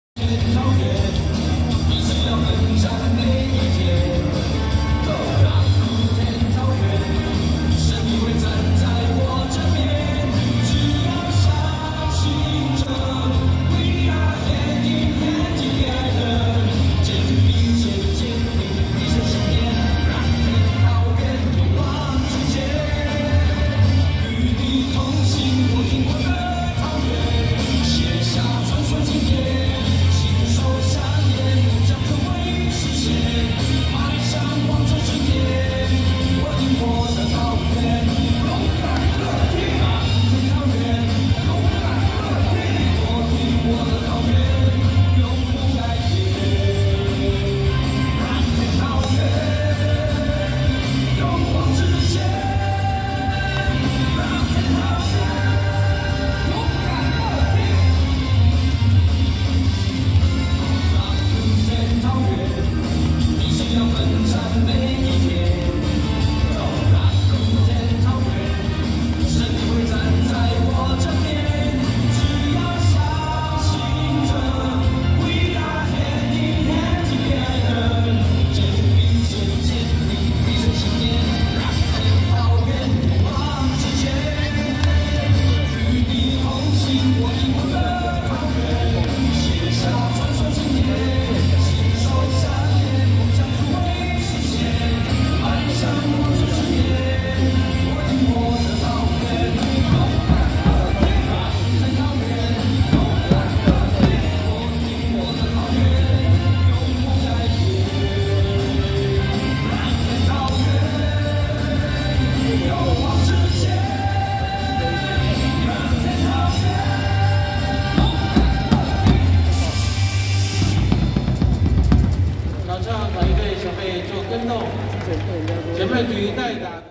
嘎琳~Yuri~陳伊 一起做個勇敢樂天女孩!! ⚾ 🦁統一獅 @ 🐒樂天桃猿.
勇敢樂天的女孩們~一起熱血挺桃猿吧!!! 😊

2023年09月28日 (星期四)⚾
🦁統一獅 @ 🐒樂天桃猿.

場地資訊:
Rakuten Monkeys 樂天桃猿
地址 : 320桃園市中壢區領航北路一段1號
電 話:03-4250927